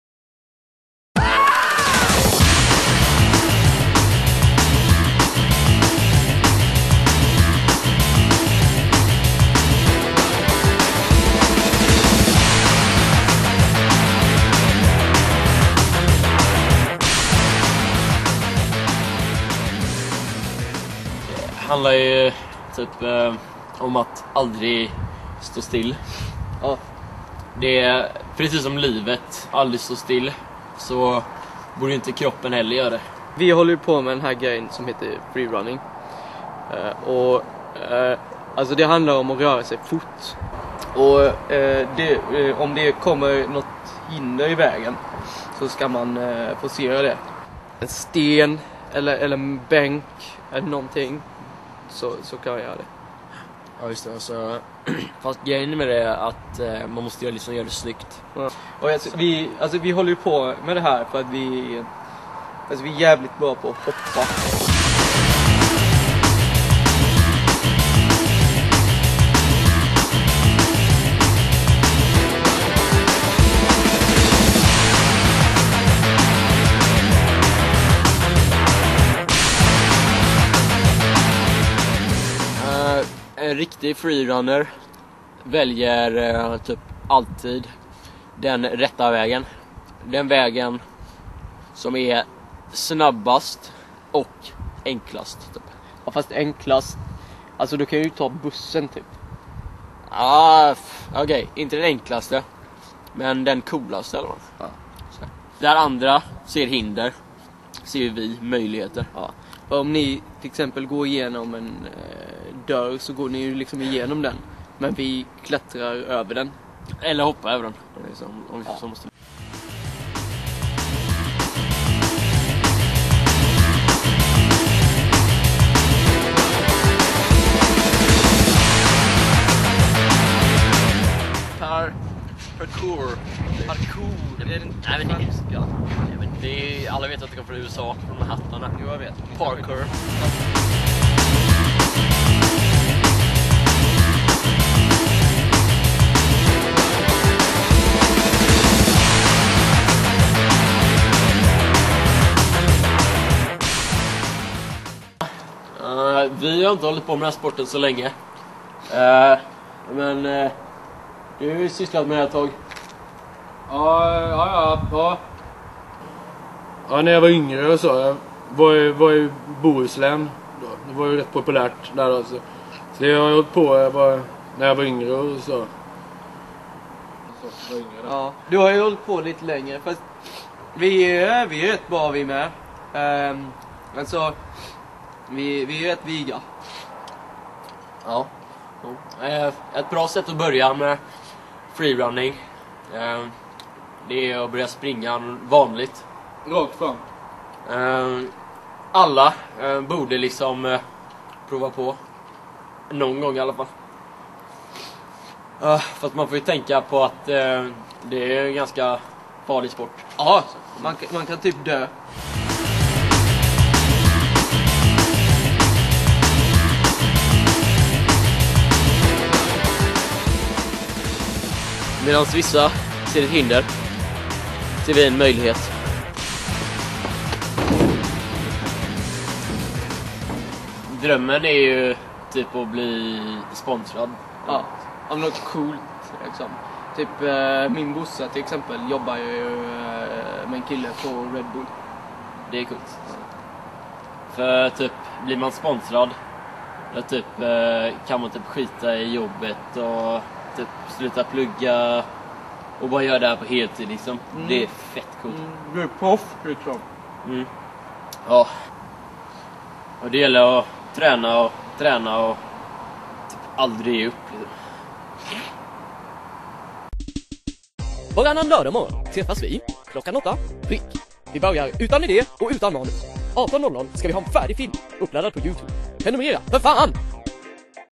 Free Running Parkour Sverige 16:9
En film om Parkour / Freerunning i Sverige. Den tredje filmen i vår serie av kortfilmer som skall skrivas, produceras och läggas upp på 8 timmar. Vi lyckades nästan.